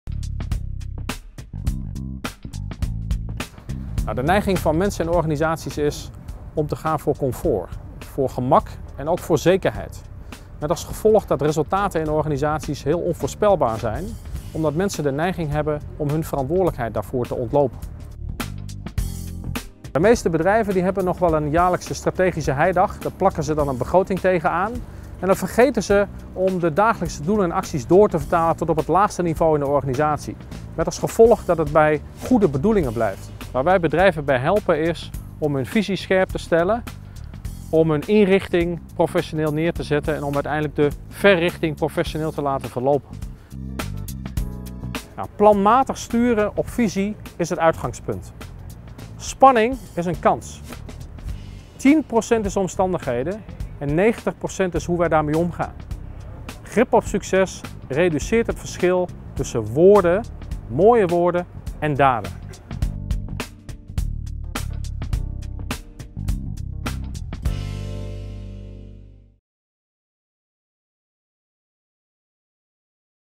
Grip op succes®
Harold Viscaal vertelt over de unieke ontwikkelingsweg Grip op succes® voor ondernemers, directeuren en leidinggevenden. Wij geloven dat 10% van ons succes wordt bepaald door omstandigheden en 90% van onze resultaten een gevolg zijn van onze mate van wendbaarheid. Grip op succes® leert ons dat succes een mindset en een methode is. Het reduceert het verschil tussen woorden en daden. Grip op succes® bestaat uit het planmatig doorvertalen van uw visie en strategie naar gerichte doelen, prestatienormen, grondhoudingen op alle niveau's van uw organisatie. Het resultaat is een structuur en cultuur van permanente wendbaarheid, een organisatie waarbij 'woorden' en 'daden' met elkaar overeenkomen, waardoor resultaten voorspelbaar worden.